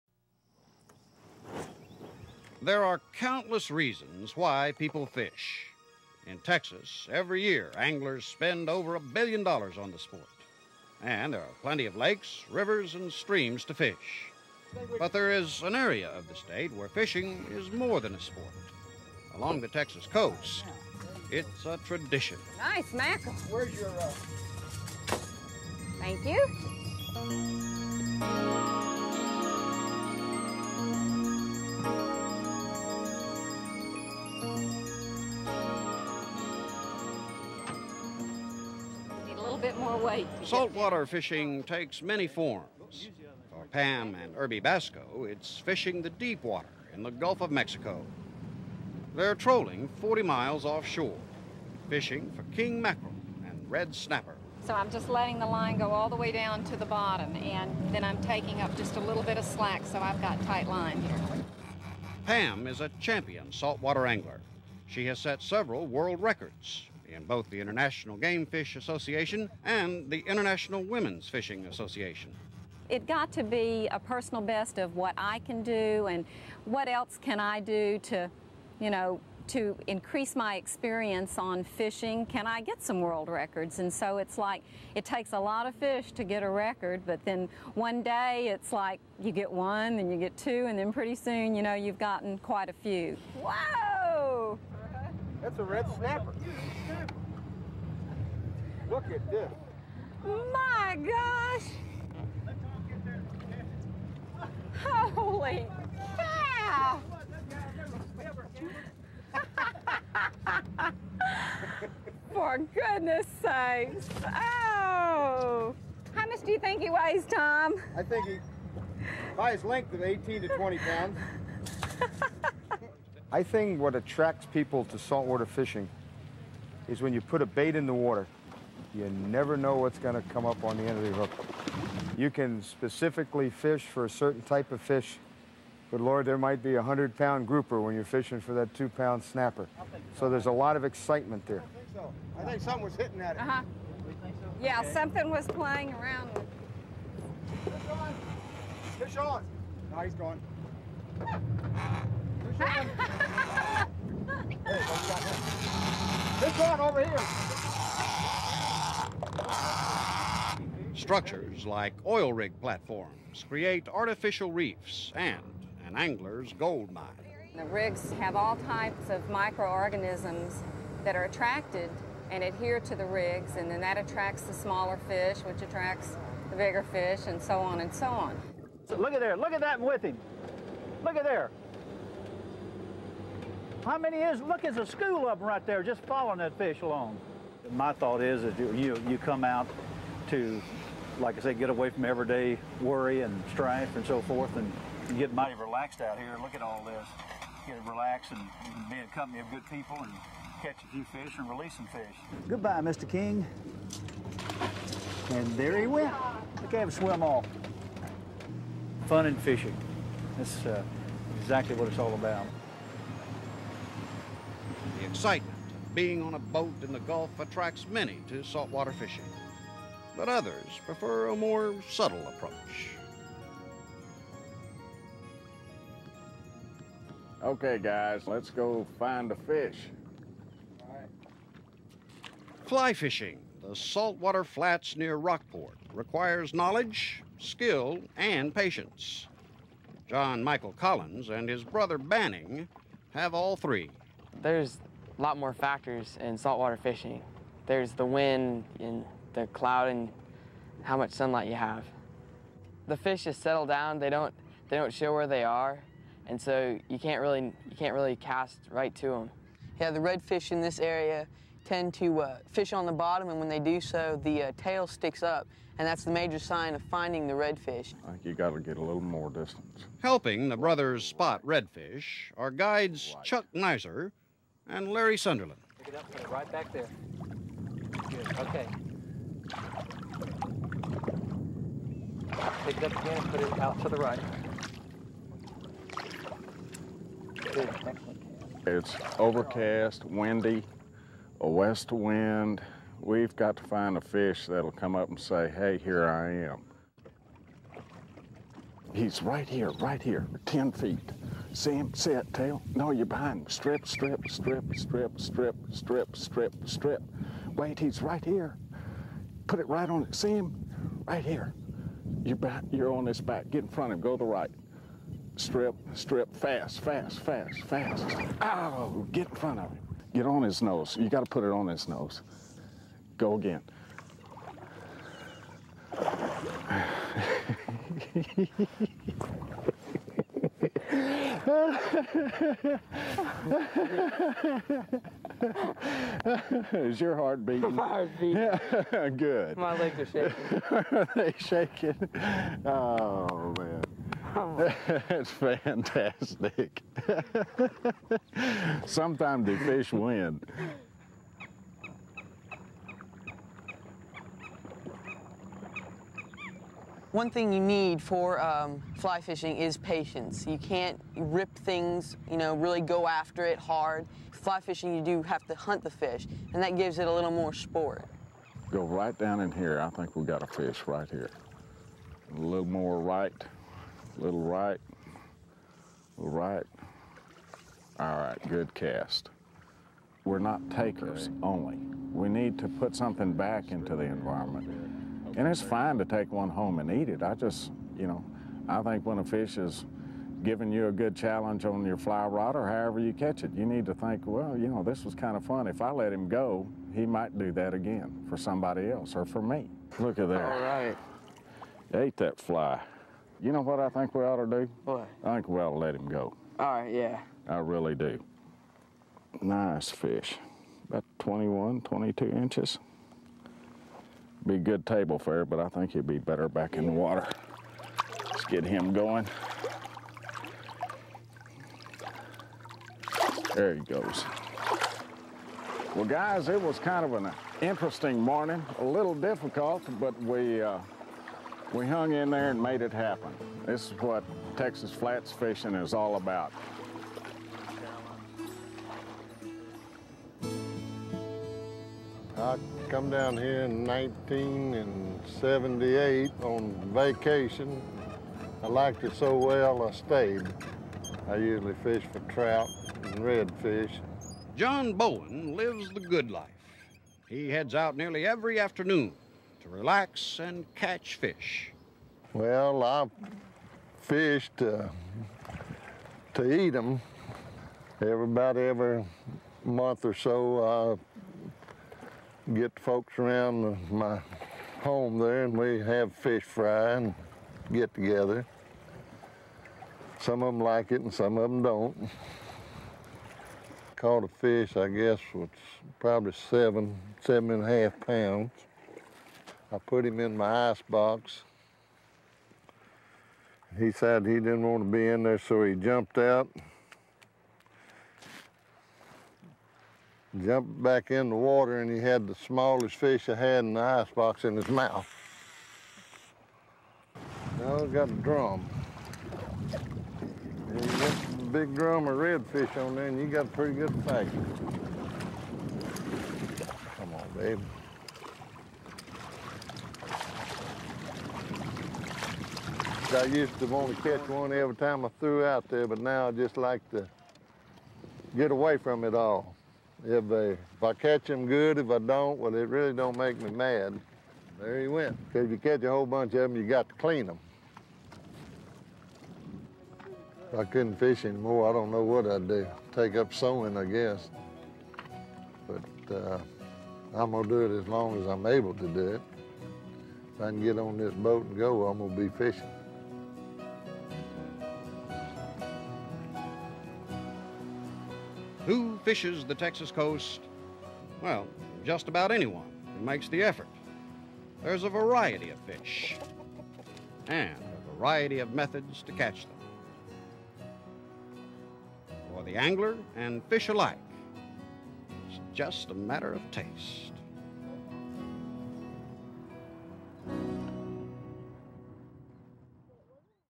Hooked on the Coast
The Texas coast is home to some of the finest sport fishing in the country. Follow along and see why anglers from all over are hooked on the coast.